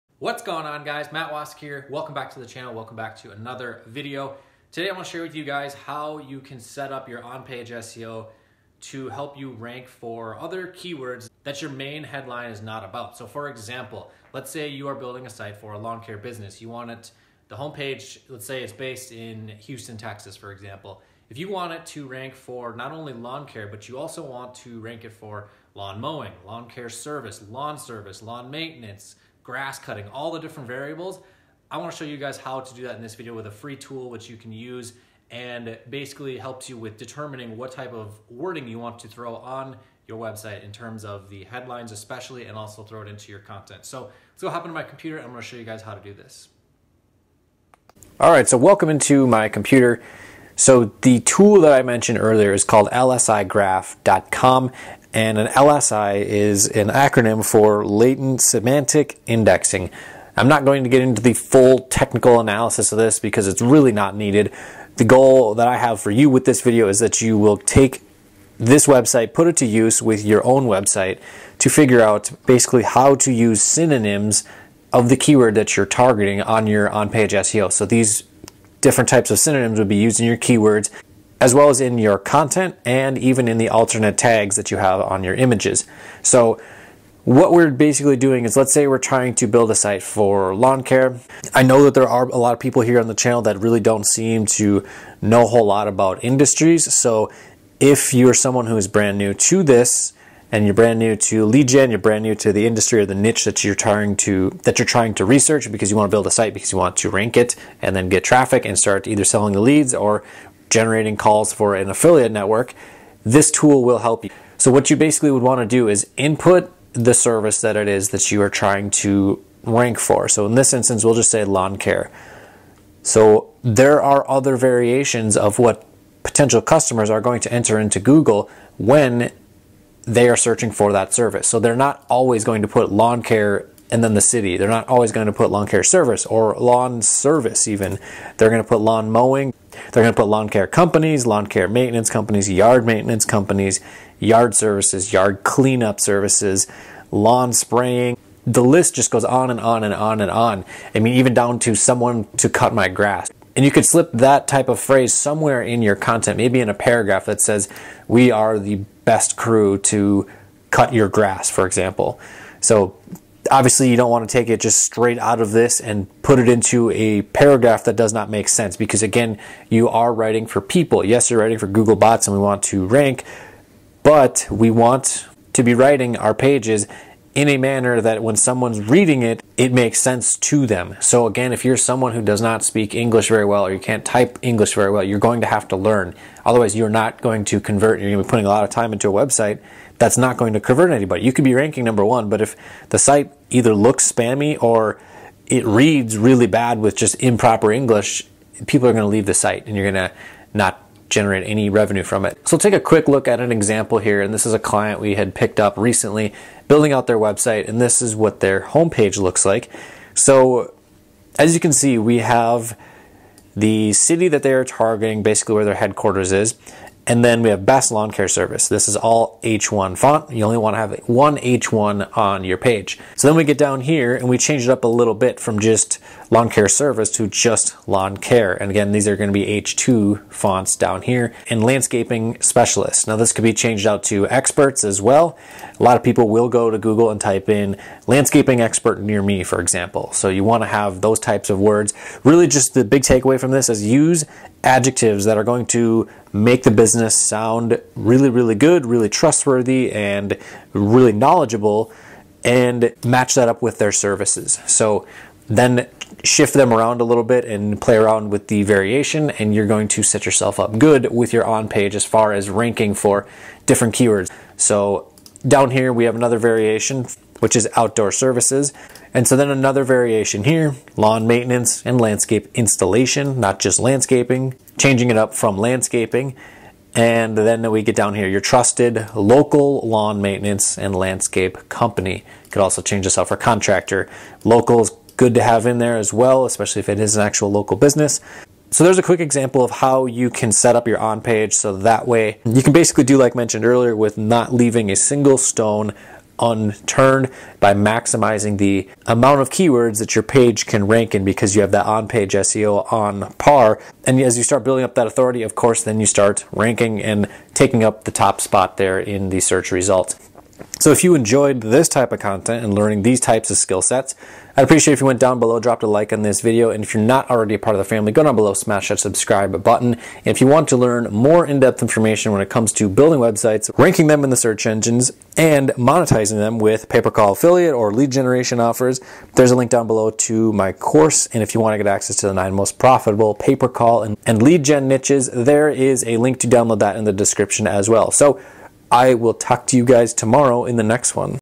How To Rank on Page 1 For Hidden Keywords Without Doing Extra Work
How to find hidden keywords and rank your site on page 1 without doing extra work.

✅ Potentially Work 1-to-1 With Matt [Build a Profitable 6-Figure Niche-Focused Digital Marketing Agency] ➤